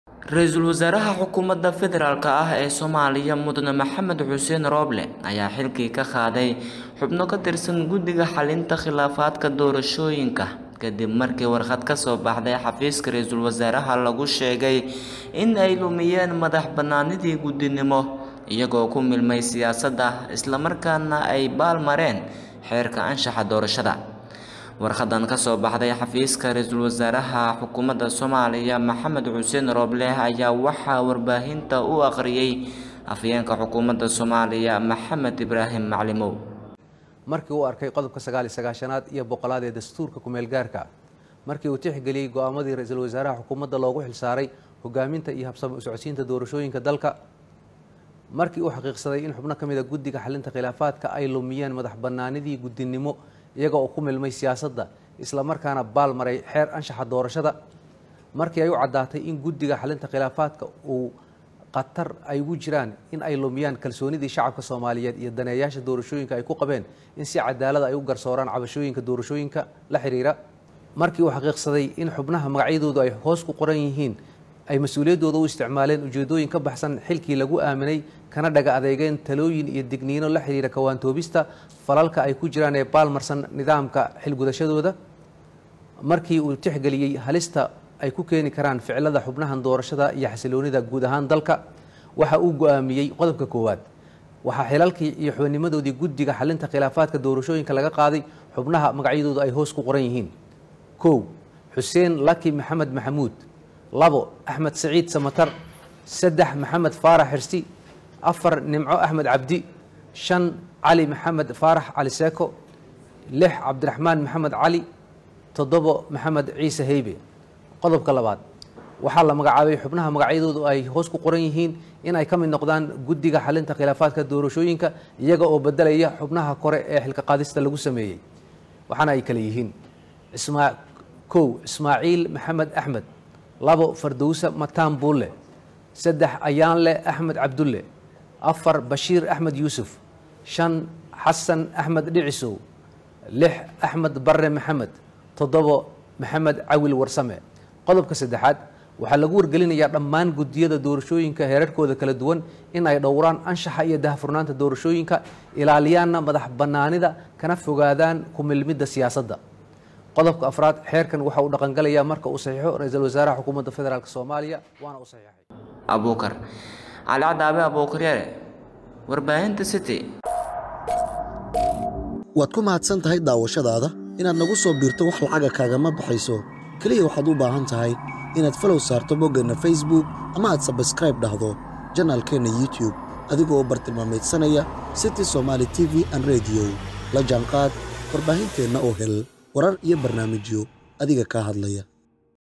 Ra'iisul Wasaare Rooble oo Xilalka ka qaaday Xubno kamid ah Guddiga Xalinta Khilaafaadka..
Ra'iisul Wasaare Rooble oo Xilalka ka qaaday Xubno kamid ah Guddiga Xalinta Khilaafaadka Doorashooyinka Soomaaliya Abukar Yare ayaa Kawaramaya